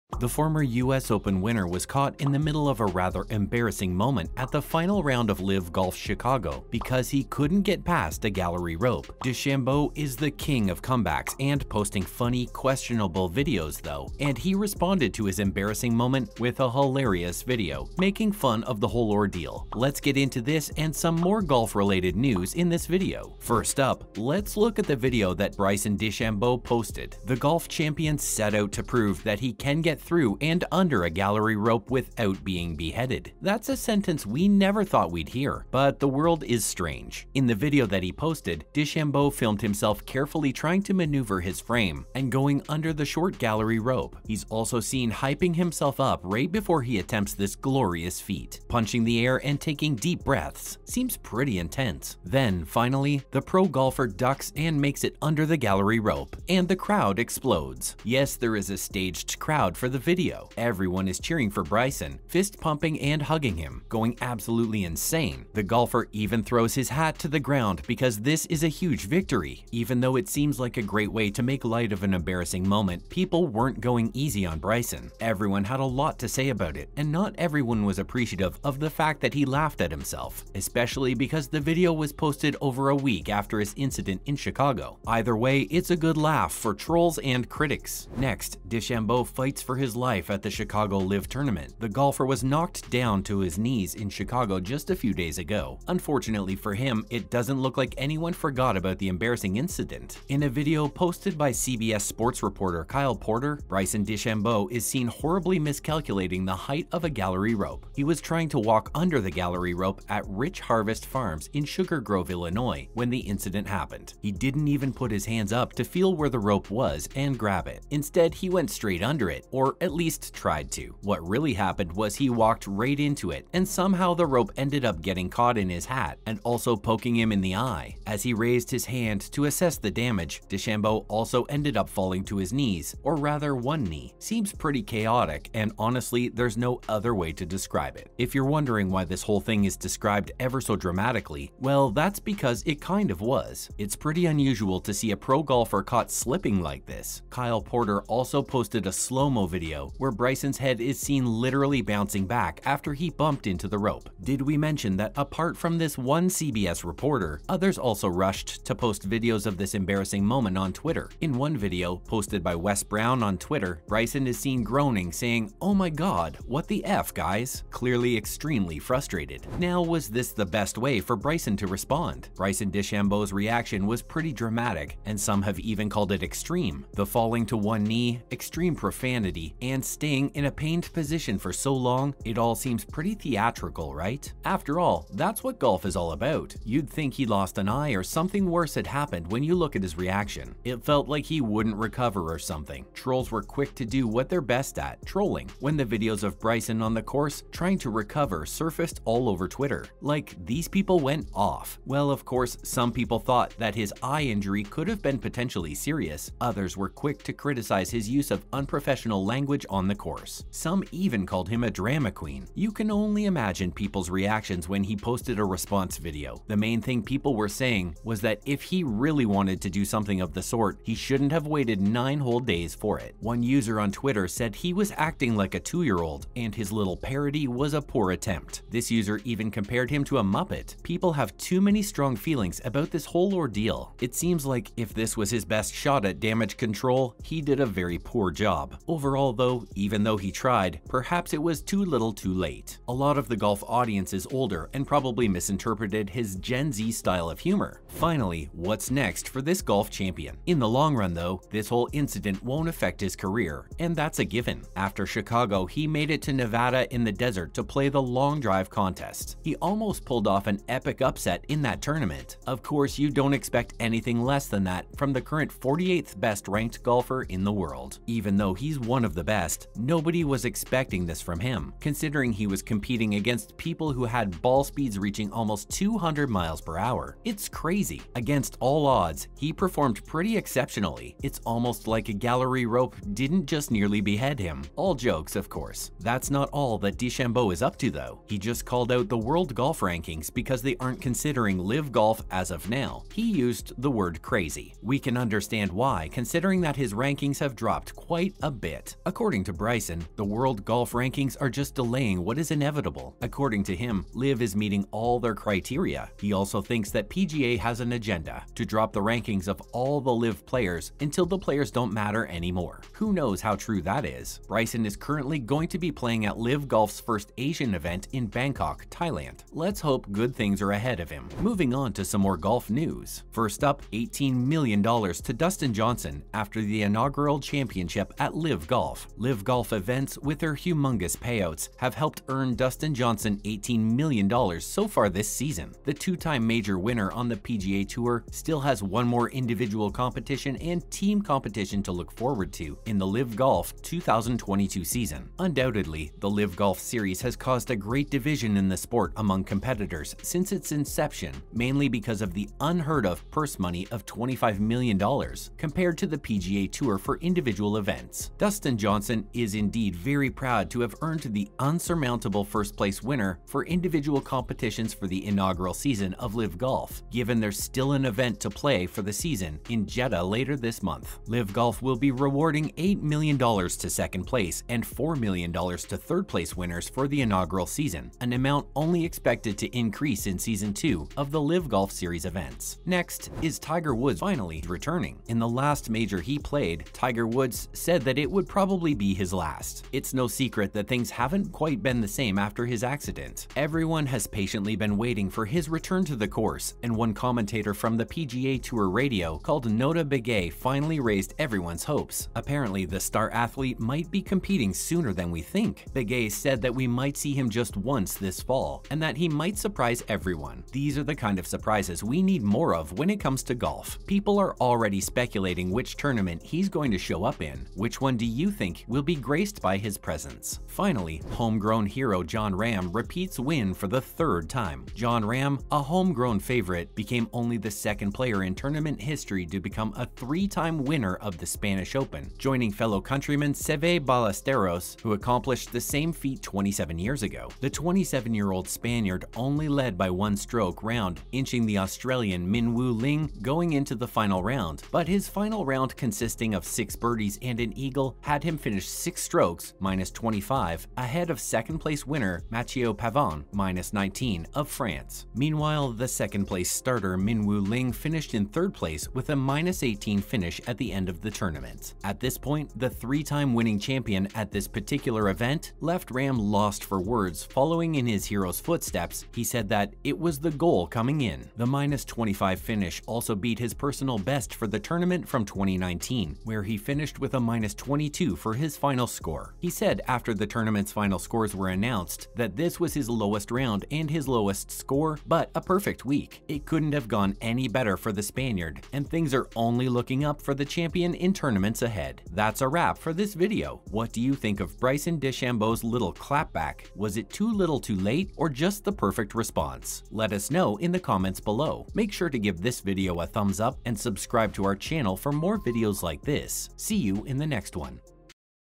Bryson DeChambeau DEFEATS Rope That Clotheslined Him..
Welcome back to Sports Zoom, today on this channel we are going to talk about “ Bryson DeChambeau released a video to prove he can duck under a rope without being beheaded” Bryson DeChambeau is known for putting out the occasional questionable/outrageous video. His latest, however, takes the cake.During the final round of LIV Golf Chicago, DeChambeau was dominated by a gallery rope. He fell to one knee and reacted like he was just hit over the middle by a linebacker.

Now, nine days later, DeChambeau has put out a video to show that he can successfully go under a gallery rope without being beheaded. It’s a tough watch. If you’re going to laugh at yourself, might be best to not wait an entire week.Check this video out and stay updated!

Bryson DeChambeau is just an insane person and doesn't know how to be funny for the life of him